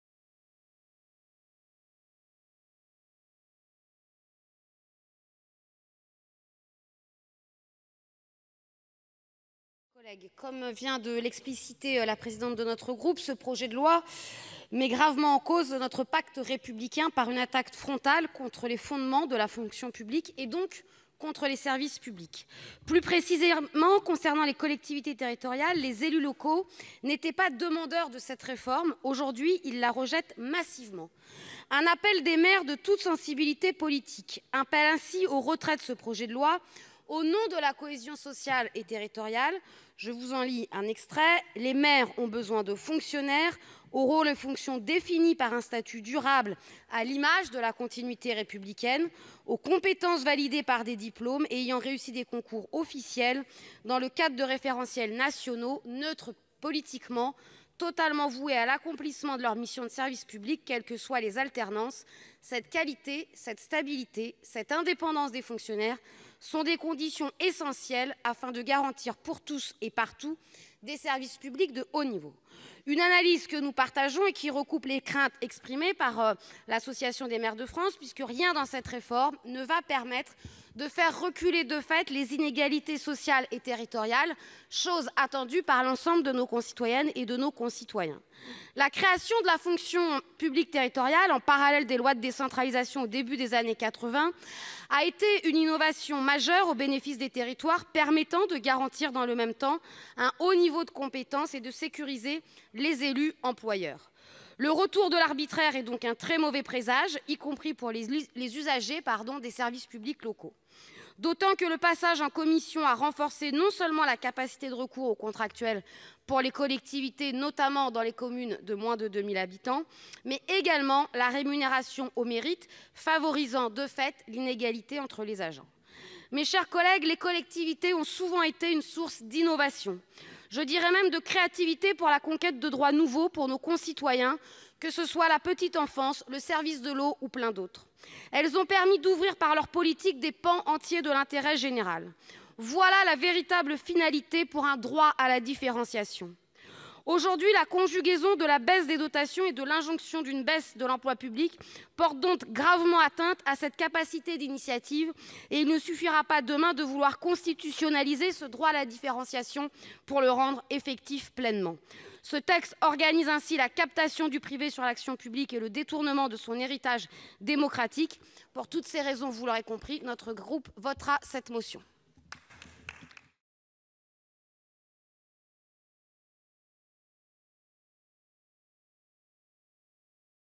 Des maires de tout bord rappellent leur attachement à des fonctionnaires régis par un statut durable
Intervention de Cécile Cukierman lors de l’examen du projet de loi de transformation de la Fonction publique : des maires de tout bord rappellent leur attachement à des fonctionnaires régis par un statut durable, recrutés par concours, neutres et voués à leur mission de service public.